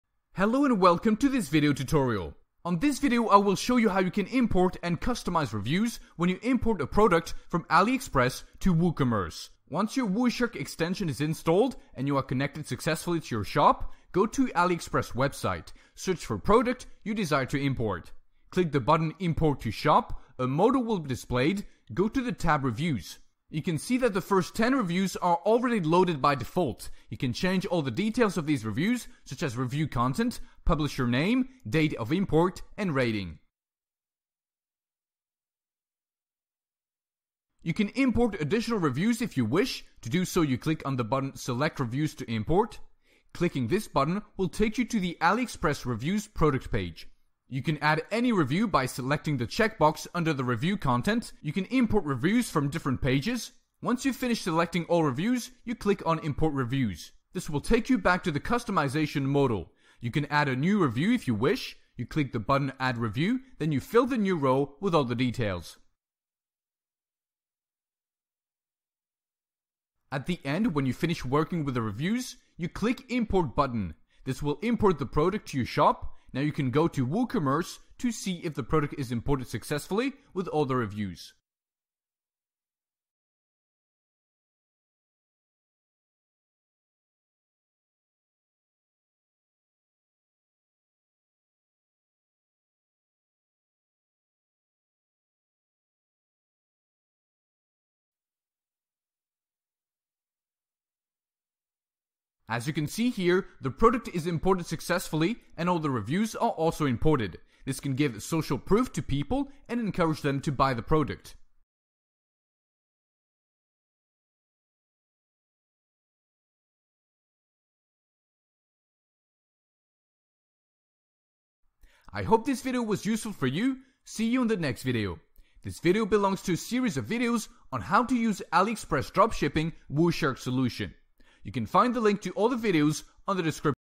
AliExpress to woocommerce import reviews
Import reviews from AliExpress to woocommerce
This video shows you how to import reviews from AliExpress products to woocommerce

this is an alternative for ryviu solution.

00:00 - Chapter 1

00:47 - Import reviews to woocommerce from aliexpress

1:20 - Choose reviews to import